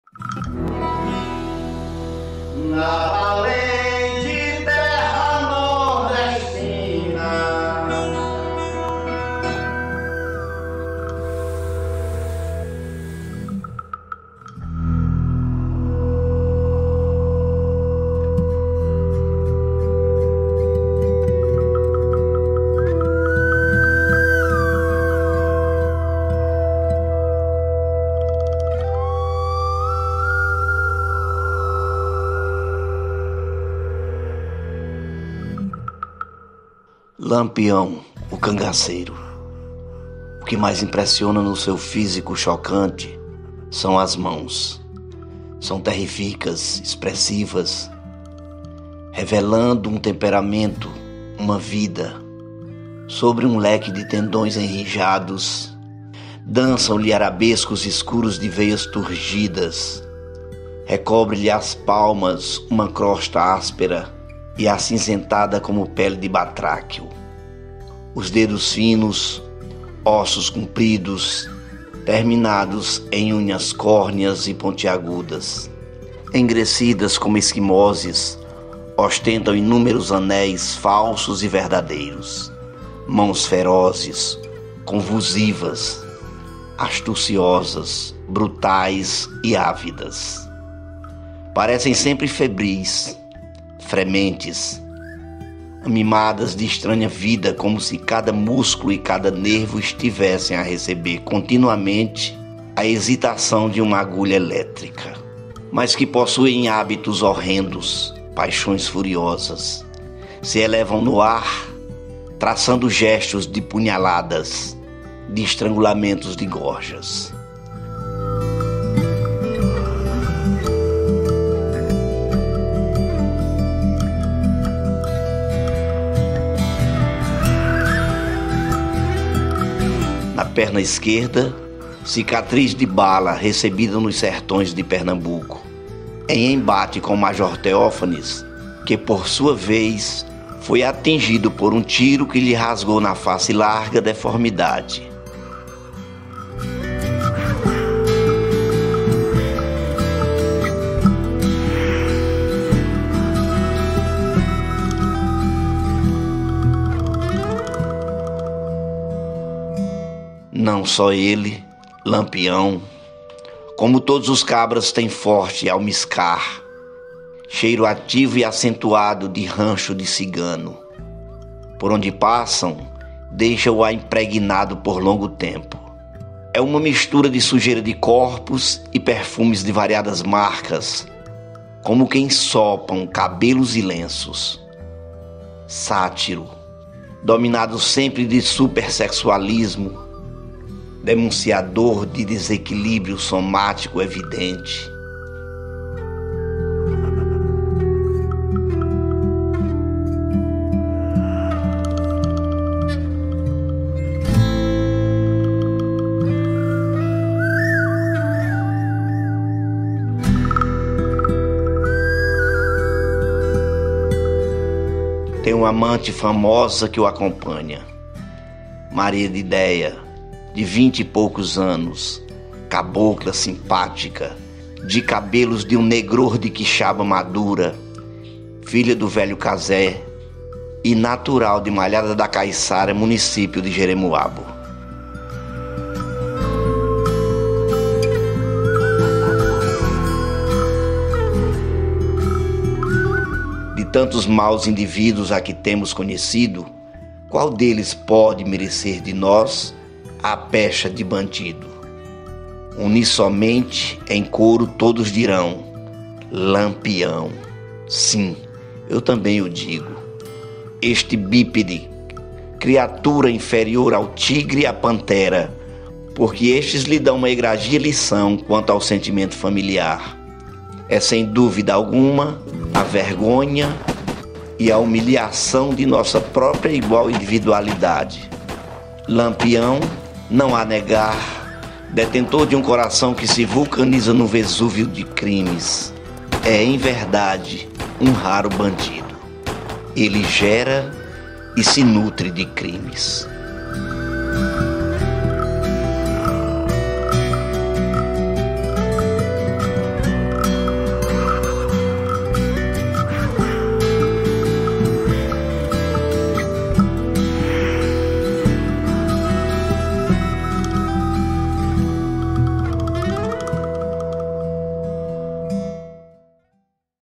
LAMPIÃO O CANGACEIRO
"O que mais impressiona no seu físico chocante são as mãos. São terrificas, expressivas, revelando um temperamento, uma vida. Extraordinariamente longas, no dorso, sobre um leque de tendões enrijados, desçam-lhe arabescos escuros de veias turgidas; recobre-lhe as palmas uma crosta áspera e acinzentada como pele de batráquio; os dedos
finos, ósseos, compridos, terminados um unhas córneas e pontiagudas, enegrecidas como equimoses, ostentam inúmeros anéis, falsos e verdadeiros. Mãos ferozes, convulsivas, astuciosas, brutais e ávidas. Parecem sempre febris, frementes, animadas de estranha vida como se cada músculo e cada nervo estivessem a receber continuamente a excitação de uma agulha
elétrica.